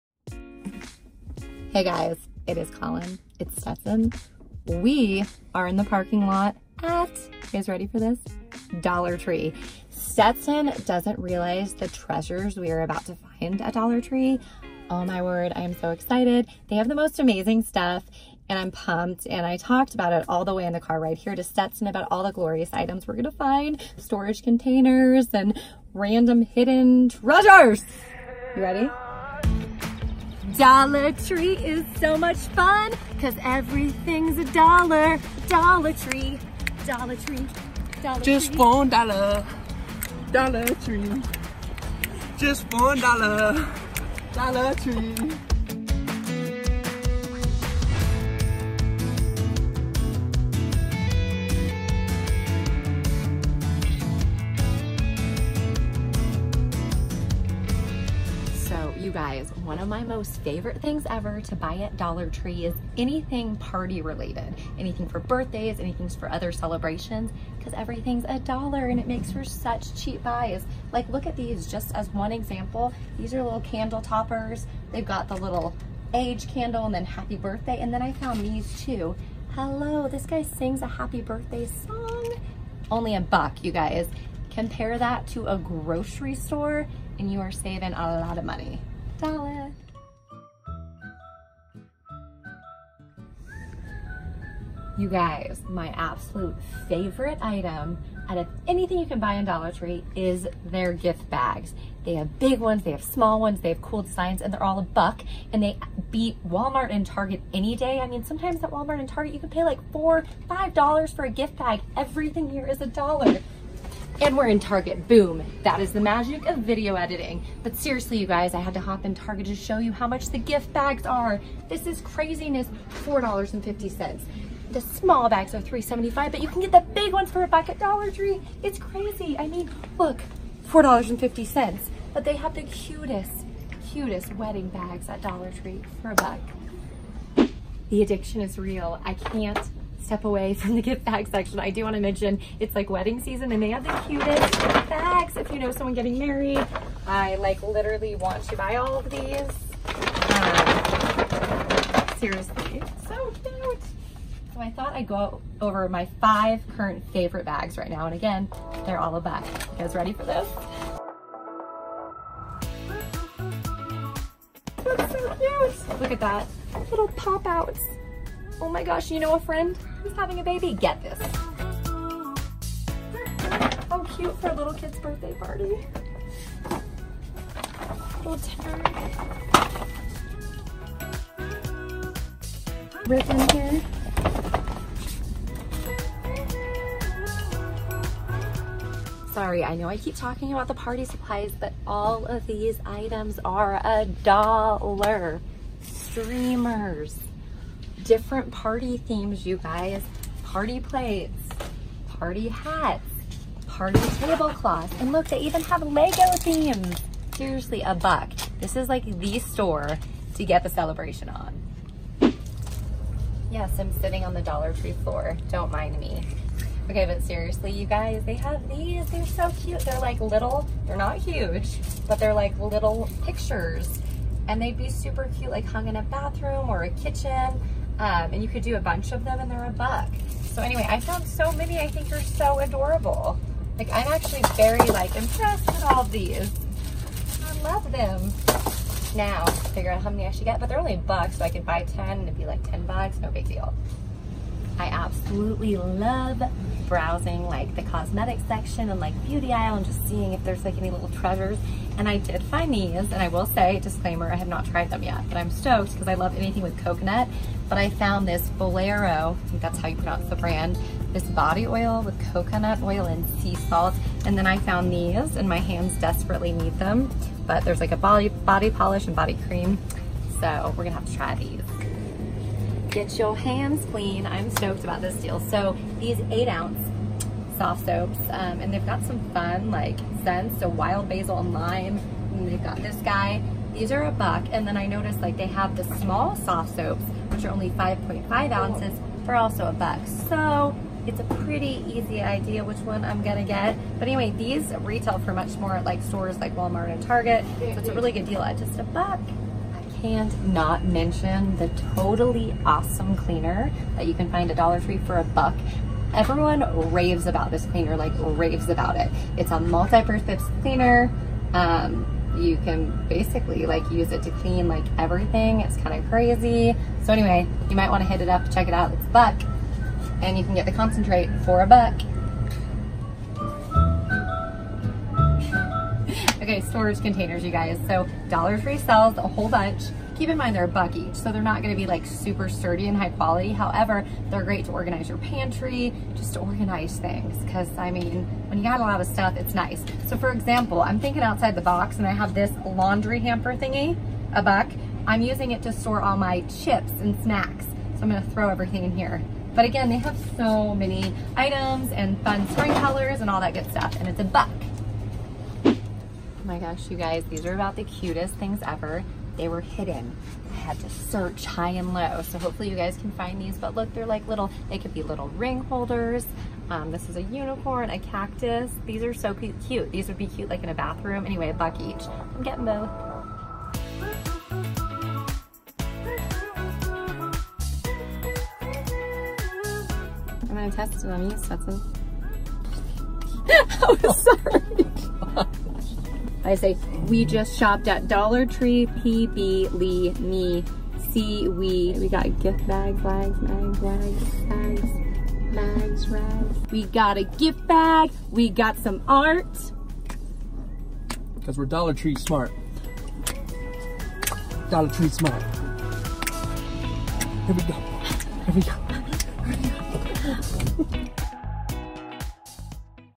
Dollar Tree Shopping for Party Supplies, Gift Bags, Home Decor & More | May 2019 | Hip2Save
Tag along with Collin and Stetson from Hip2Save as they find fun, frugal items in the party section at Dollar Tree. Collin also finds cute gift bags, home decor, storage containers, and little trinkets in store as well!

Love Stetson's shirt?